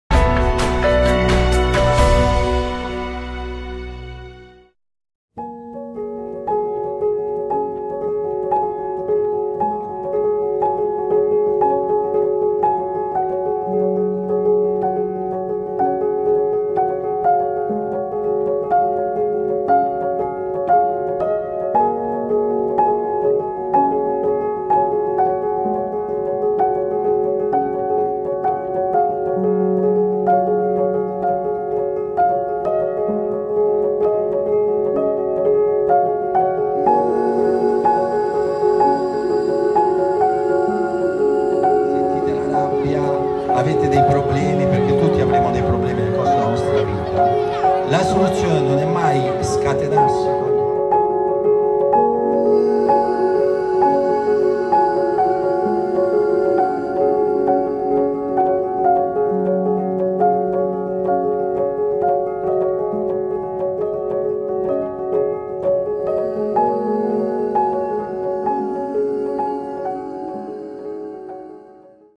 Un corteo per dire "No" alla violenza di genere
I ragazzi delle scuole cittadine hanno sfilato per le vie del centro